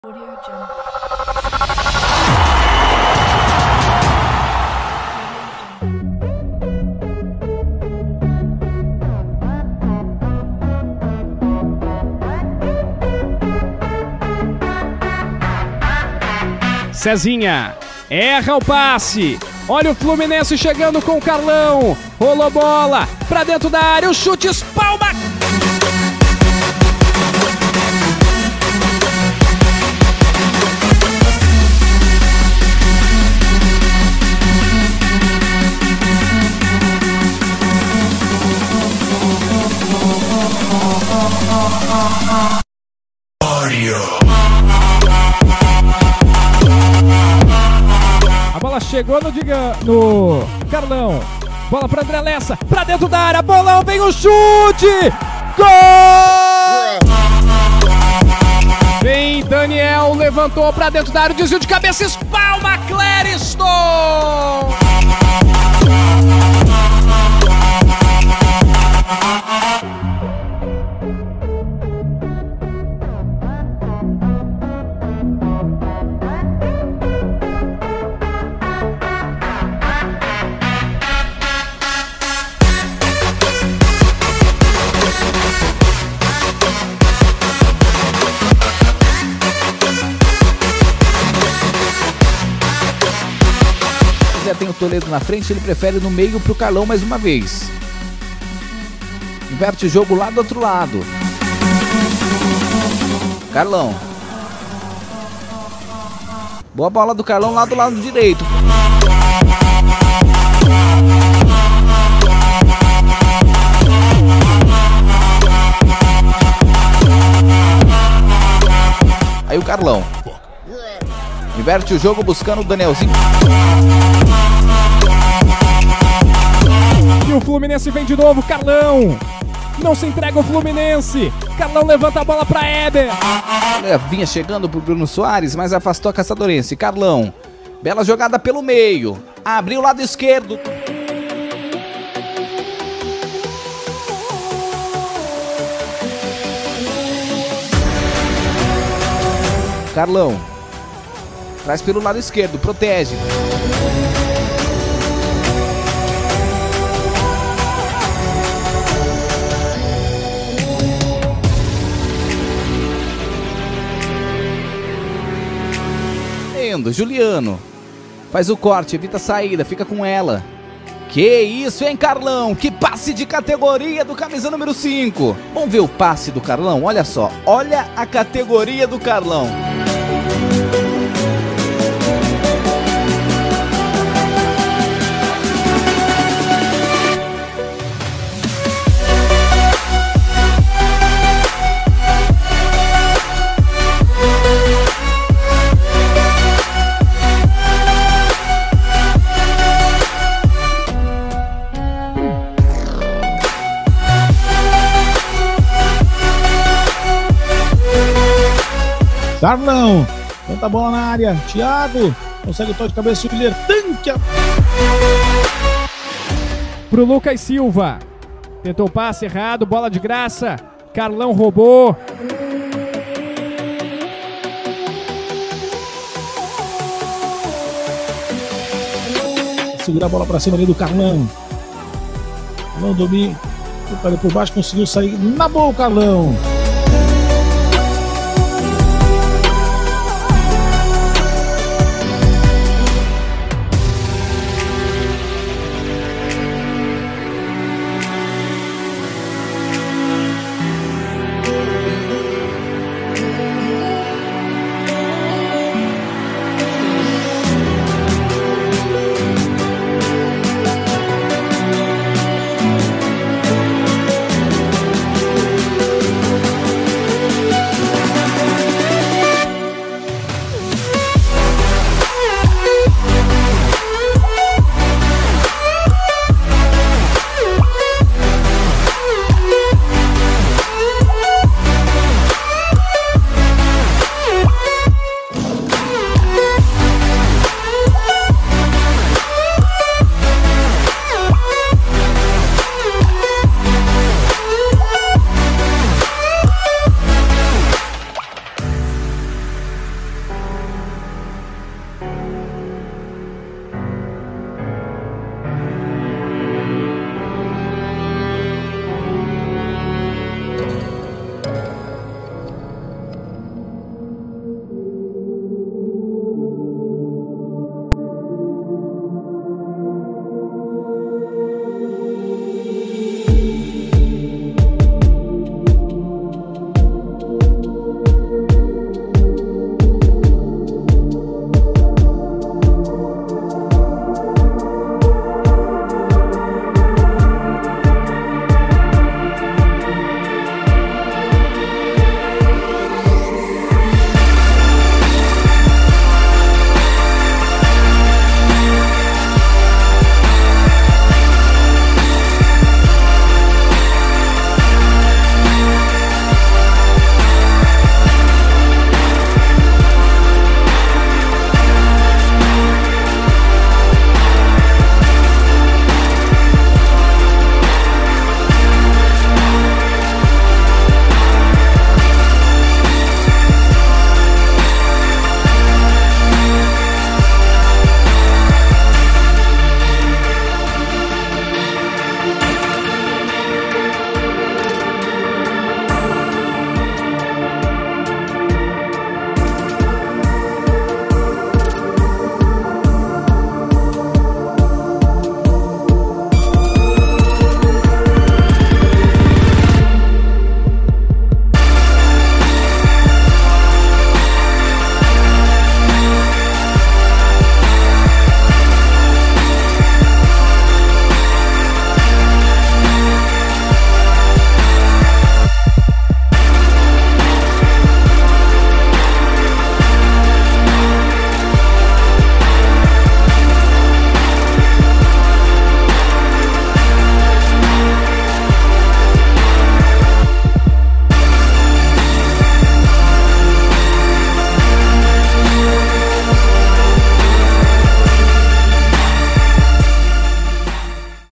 CARLÃO - VOLANTE/LATERAL-ESQUERDO - FLUMINENSE-SC 2021
Melhores momentos do atleta Carlão, volante do Fluminense-SC. Siga ele no instagram: @carlaao_97